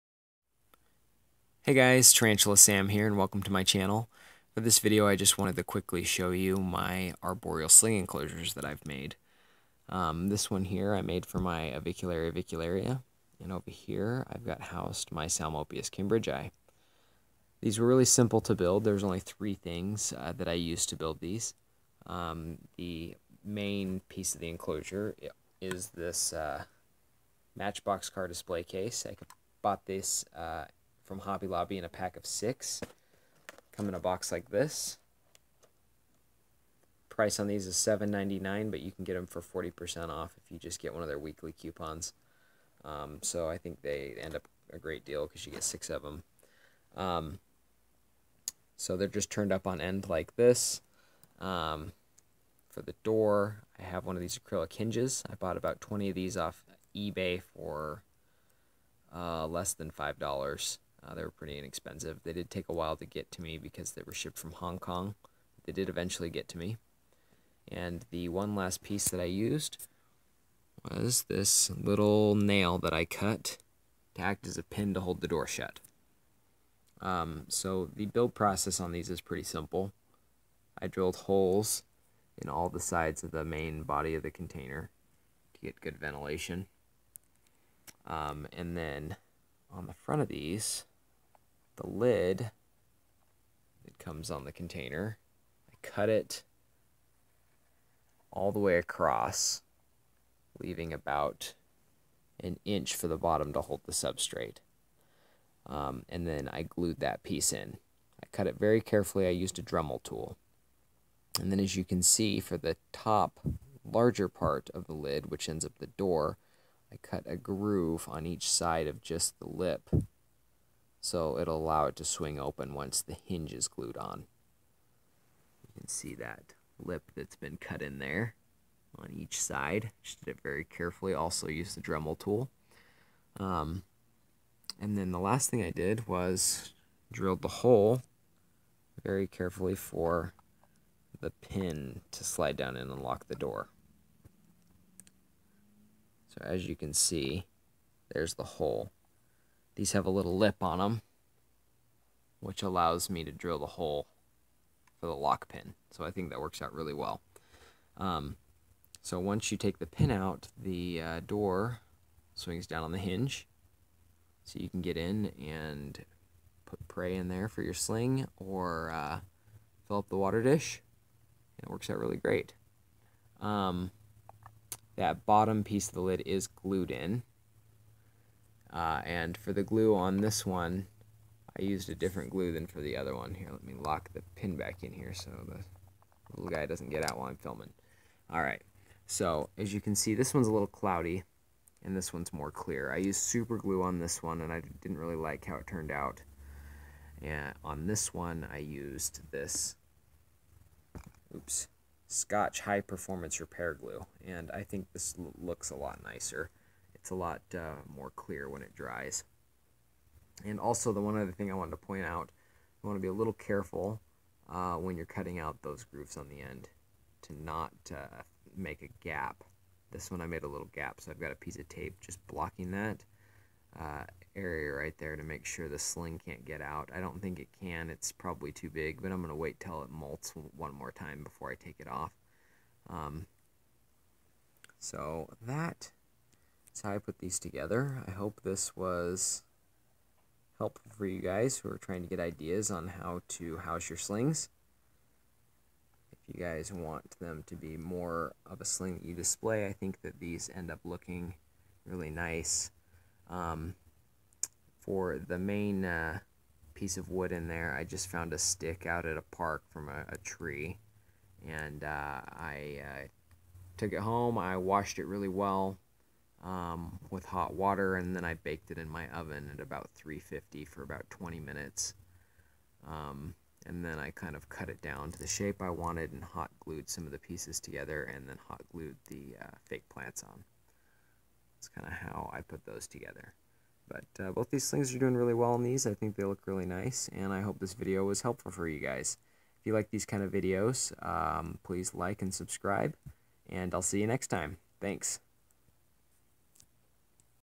Arboreal Sling Enclosures: Hobby Lobby Display Cases
I made these arboreal sling enclosures from Hobby Lobby display cases.